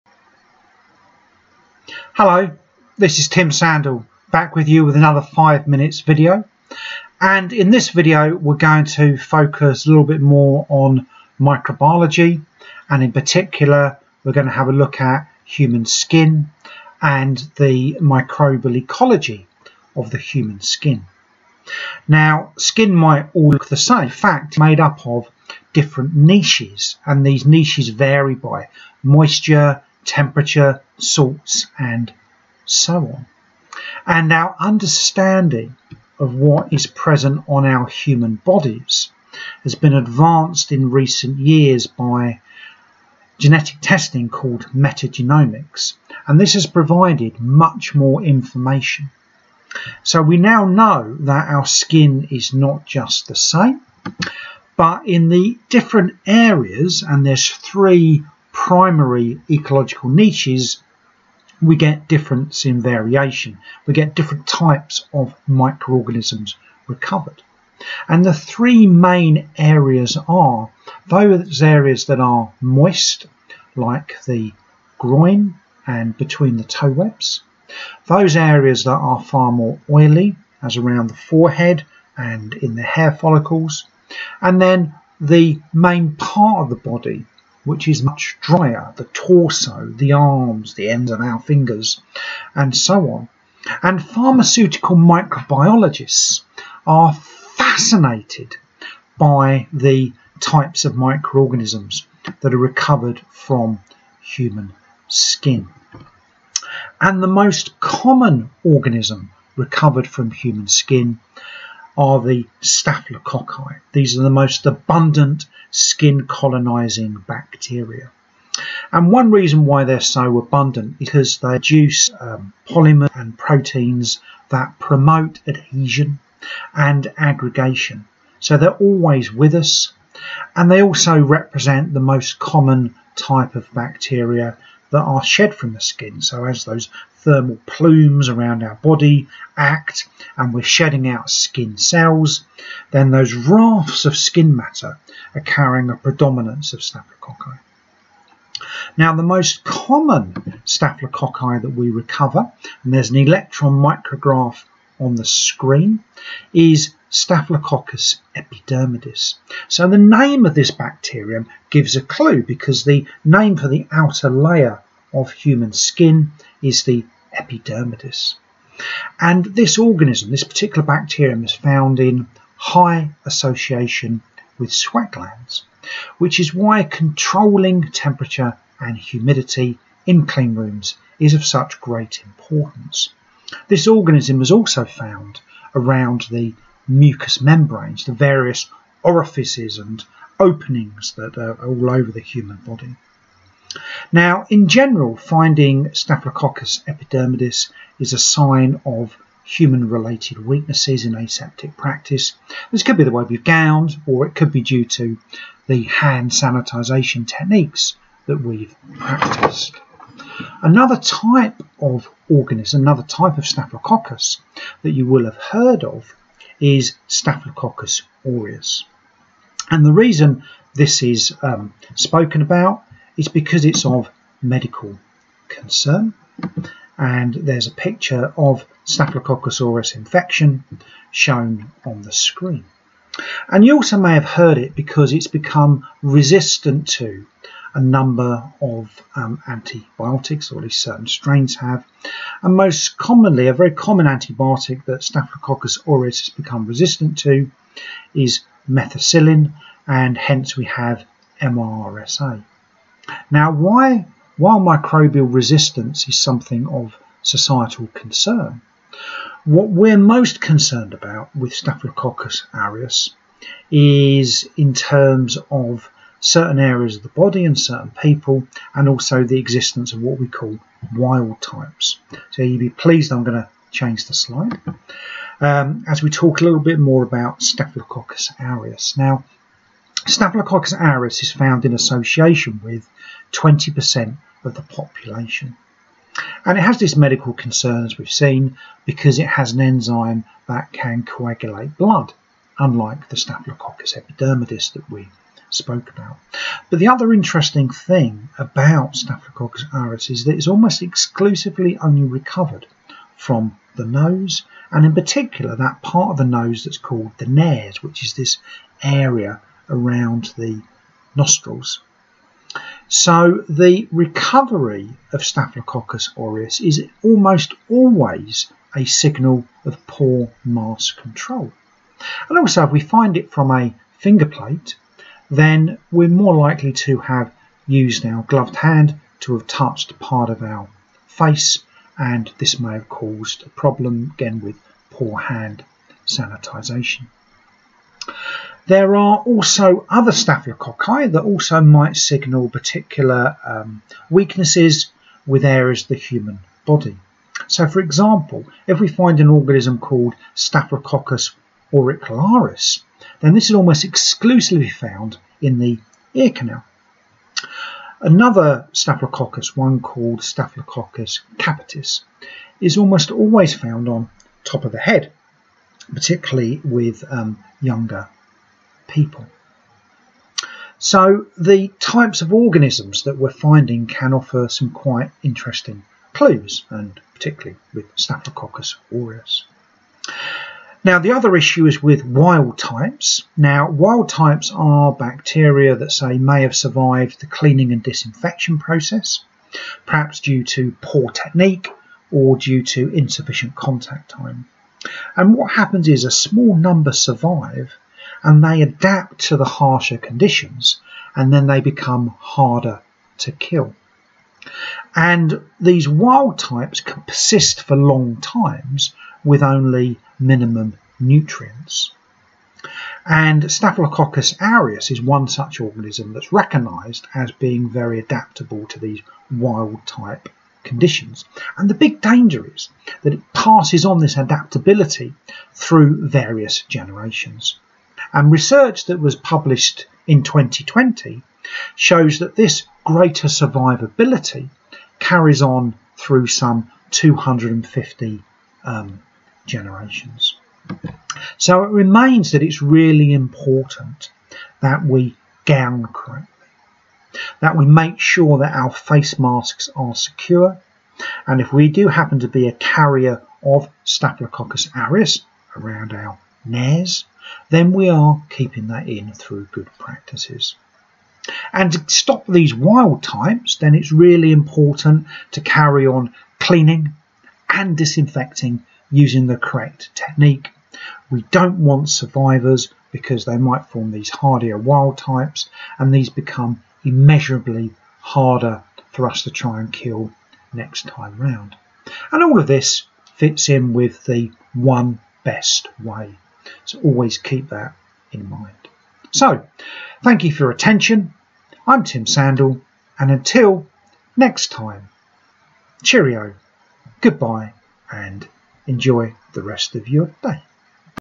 Common bacteria and the human skin microbiome
What the recovery of different bacteria from the skin means and what this means for cleanroom controls.